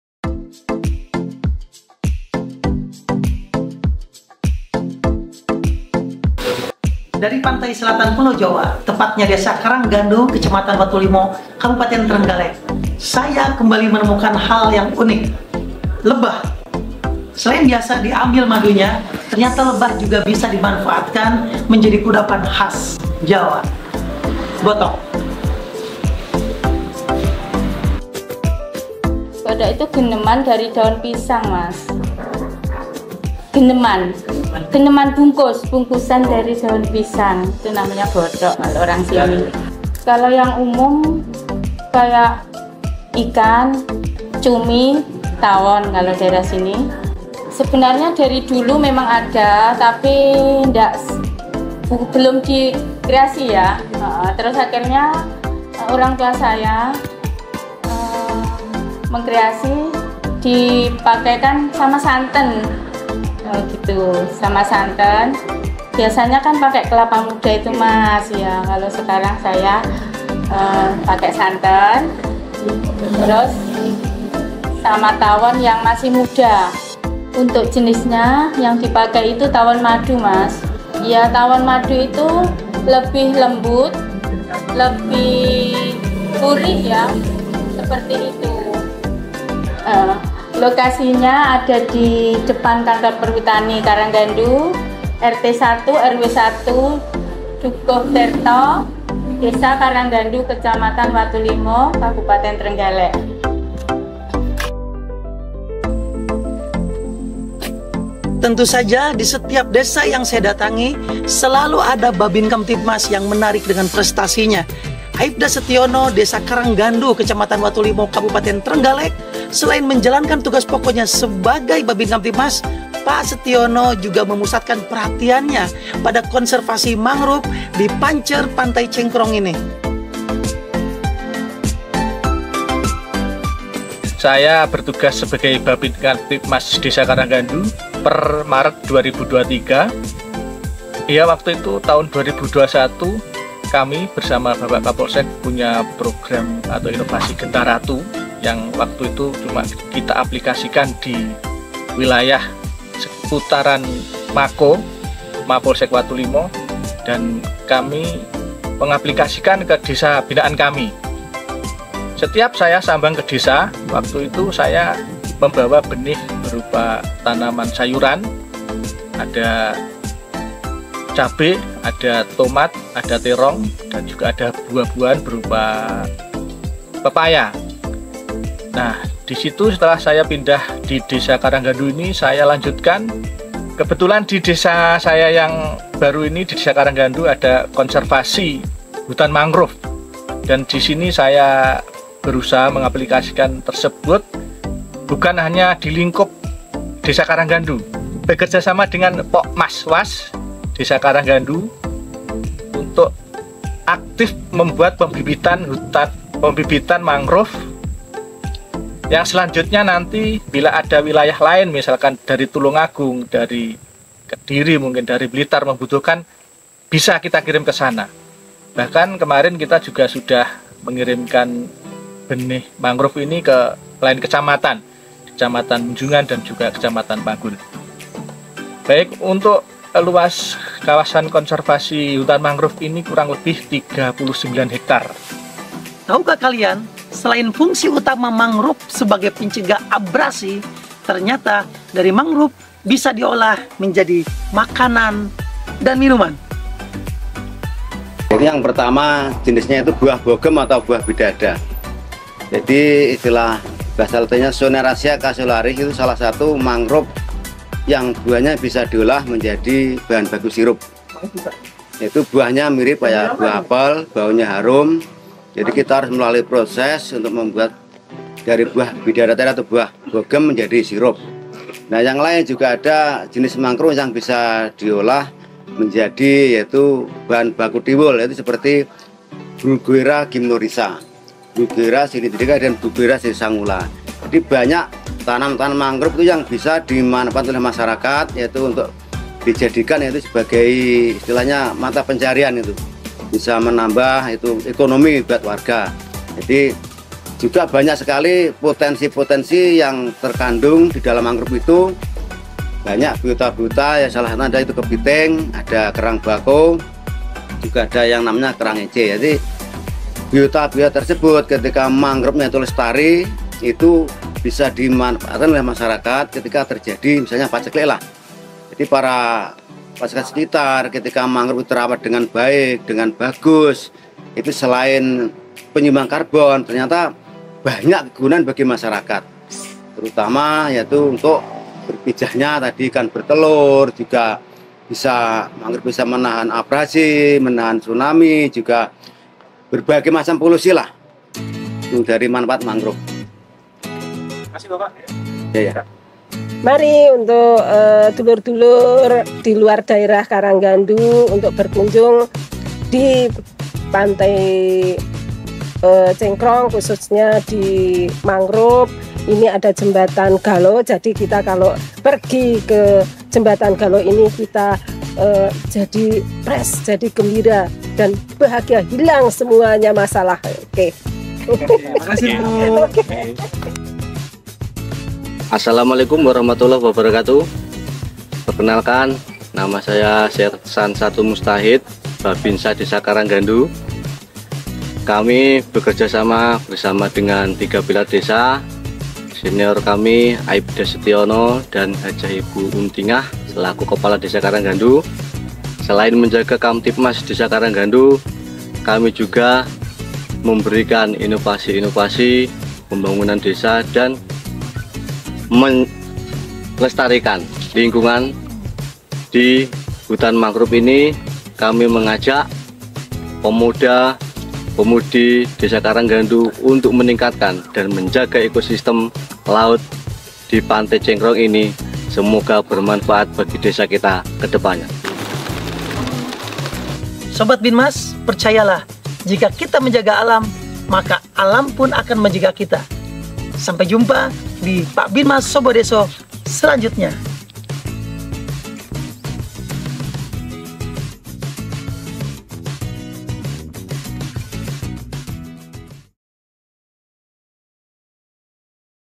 Pak Bhabin Karanggandu, Potret Polisi Baik Penjaga Alam Tetap Lestari | Pak Binmas Sobo Deso
Di video kali ini, kita akan mengangkat kisah menakjubkan seorang "Polisi Baik" yang tidak hanya melindungi keamanan, tetapi juga memotivasi masyarakat Desa Karanggandu, Kecamatan Watulimo, Kabupaten Trenggalek, Provinsi Jawa Timur, untuk menjaga kelestarian hutan mangrove.

Pak bhabin ini bukan hanya seorang penegak hukum, tetapi juga pahlawan lingkungan yang telah memutuskan untuk berkontribusi dalam menjaga ekosistem yang sangat berharga. Dengan kepedulian dan komitmennya terhadap alam, Pak Bhabin telah memimpin inisiatif untuk melibatkan masyarakat setempat dalam upaya pelestarian hutan mangrove.

Dalam video ini, kita akan menyaksikan bagaimana Pak bhabin bekerja sama dengan warga setempat dalam penanaman bibit mangrove, membersihkan pantai, dan menyelenggarakan kegiatan pendidikan lingkungan. Dari wawancara dengan Pak Polisi dan warga, kita akan mendapatkan pemahaman mendalam tentang pentingnya menjaga kelestarian mangrove untuk keseimbangan ekosistem dan melindungi lingkungan sekitar.

Mari kita bersama-sama menginspirasi dan mendukung upaya Pak Polisi Baik ini dalam melestarikan hutan mangrove di Desa Karanggandu.

Terima kasih telah bergabung dengan kami dalam perjalanan melindungi alam dan menciptakan masa depan yang berkelanjutan!